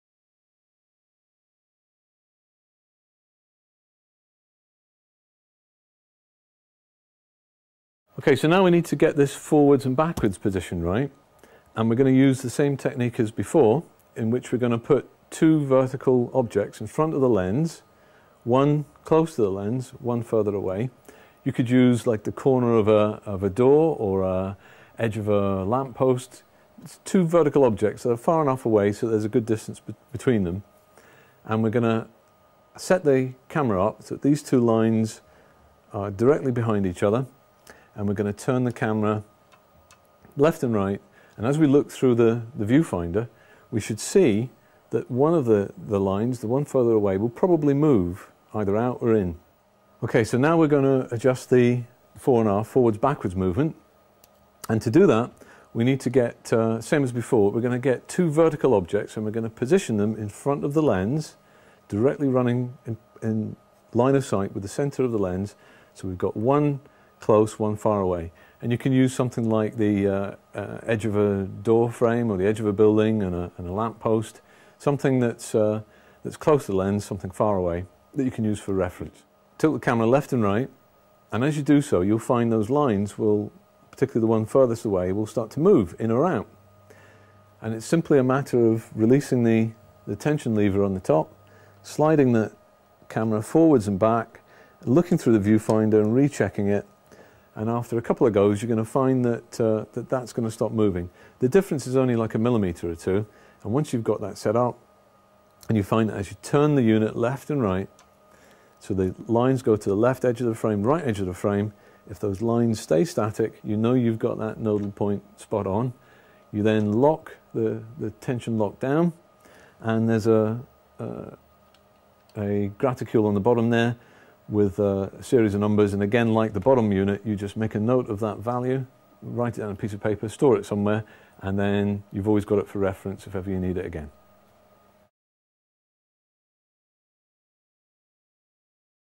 The manfrotto QTVR with bogenimaging & 360 Tactical VR [pt6]
part6 in our quicktime vr series with bogen imaging and 360 tactical vr showing us how to use manfrotto products.